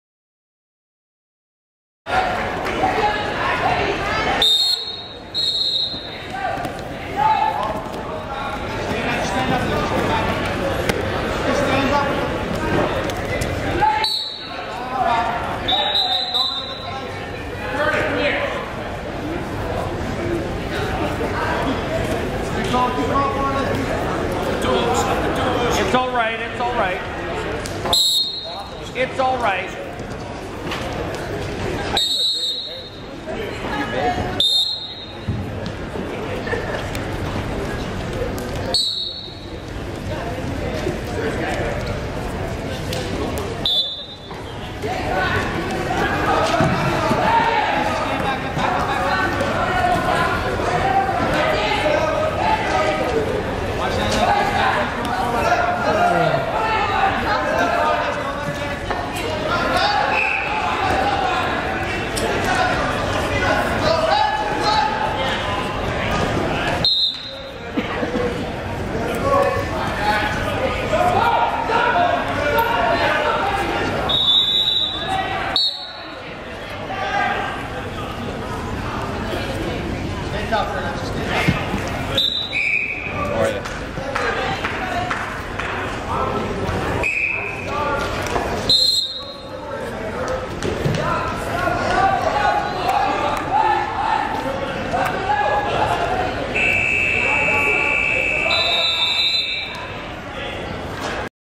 2013 Guelph Open: 48 kg Jasmine Slinn vs. Liz Wigle
2013 Guelph Open 48 kg Match Between Jasmine Slinn (Cattown Wrestling Club) and Liz Wigle (Queens University)